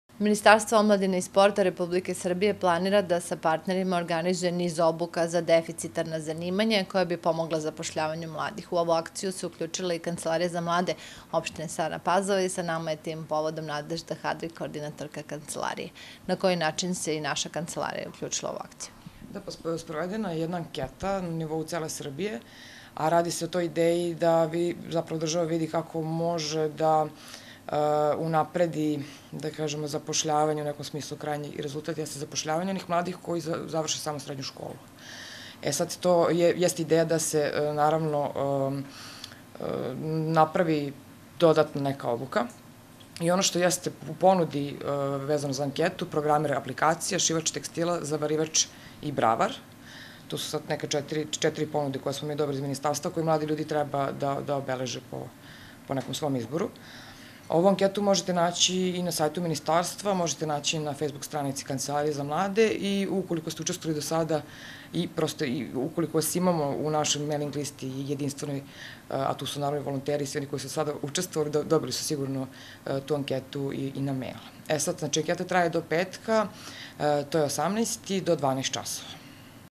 Deficitarna zanimanja
Kancelarija za mlade Opštine Stara Pazova učestvuje u anketi o deficitarnim zanimanjima.